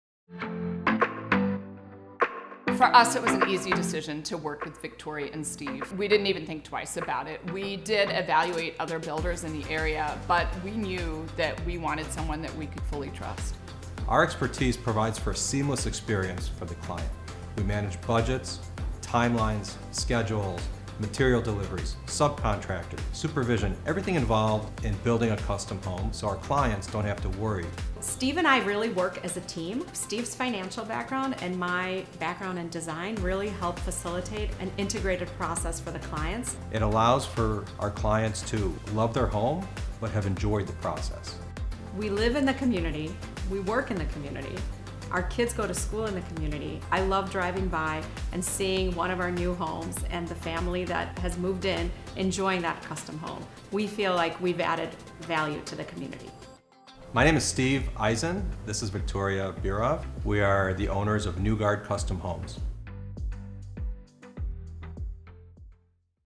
About Newgard Custom Homes, Short Version
At Newgard Custom Homes, we are committed to creating a new type of luxury residential construction company.  We are a boutique luxury home builder.  We choose to focus on a handful of projects at a time, giving us the opportunity to stay actively and personally involved in each project.  Through every step of the process, we work directly with clients, architects, interior designers – ensuring that every detail is carefully planned and constructed in a clean, efficient, beautiful and cost efficient manner.  We listen and transform your vision into the home that you will cherish for years to come. Our focus is on the needs of your family and the requirements of your lifestyle. We put our clients first. Our goal is to build a long lasting relationship with you by translating your ideas into a beautiful home that expresses your personality and style.